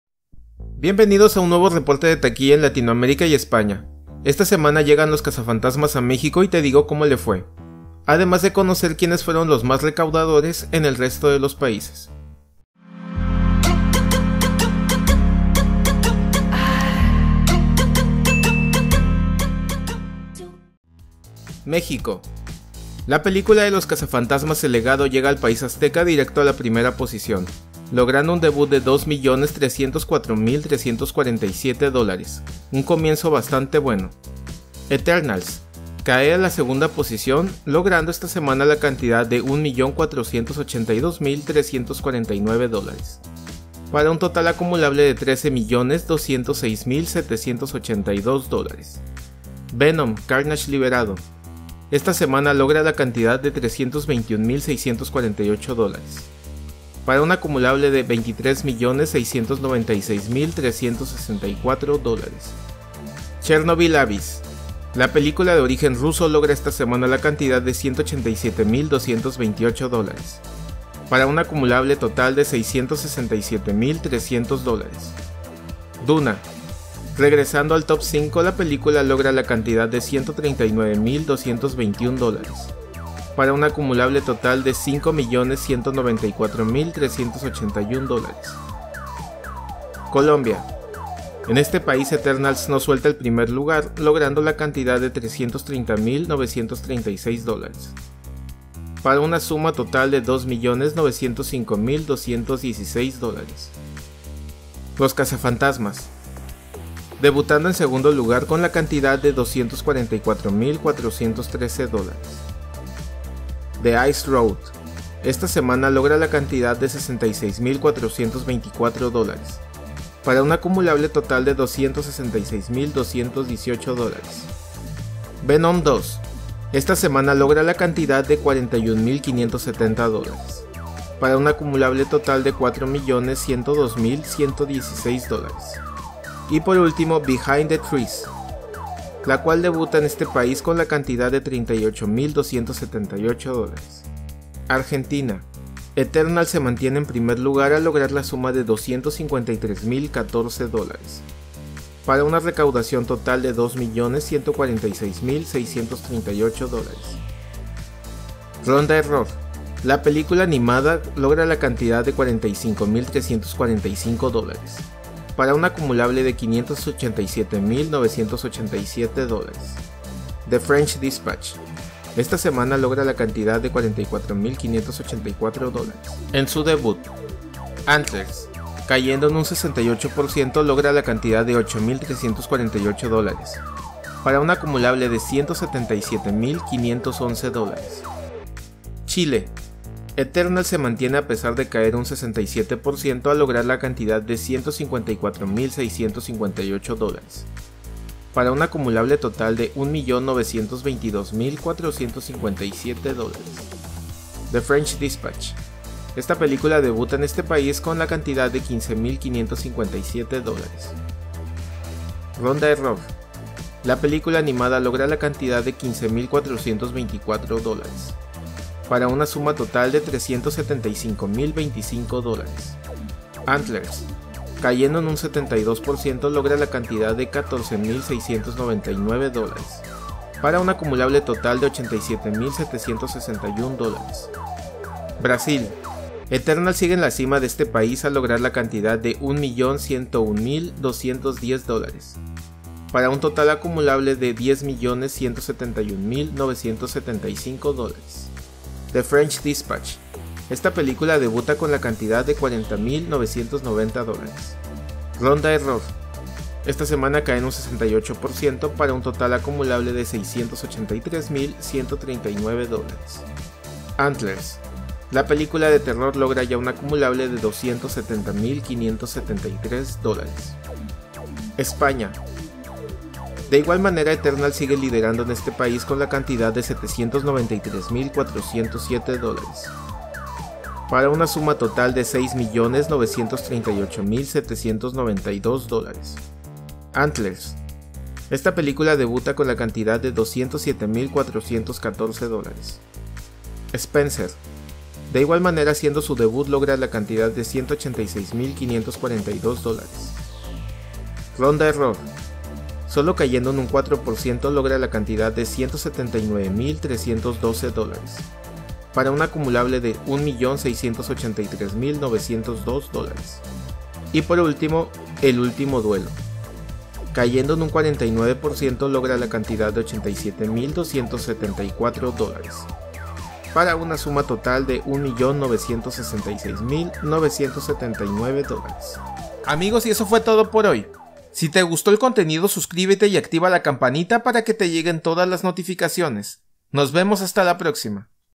Cazafantasmas logra 2 millones en mexico,eternals sigue liderando en la gran mayoria de paises
esta semana en taquilla latinoamerica: Los cazafantasmas / Ghostbusters es la película mas taquillera en mexico, eternals sigue liderando en colombia, argentina, españa, brasil y chile, venom   sigue estable en algunos paises, Chernóbil  entre las 5 peliculas mas taquilleras en mexico, antlers y spencer debutan en españa y the french dispatch debuta en brasil, argentina y chile.